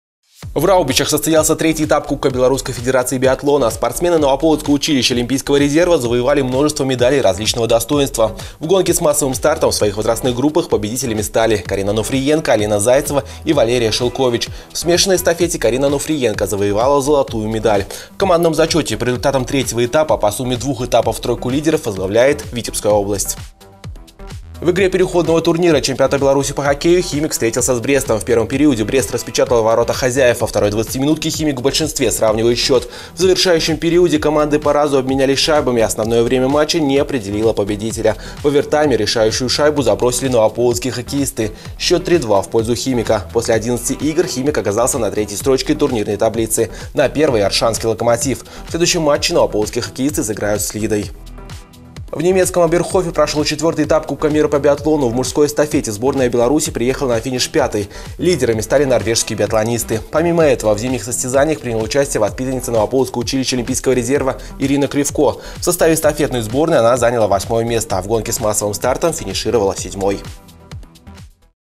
Спортобзор
Этап кубка Белорусской федерации биатлона, игра переходного турнира чемпионата Беларуси по хоккею, этап кубка мира по биатлону. О результатах этих событий далее в спортивном обзоре Глеба Прокофьева.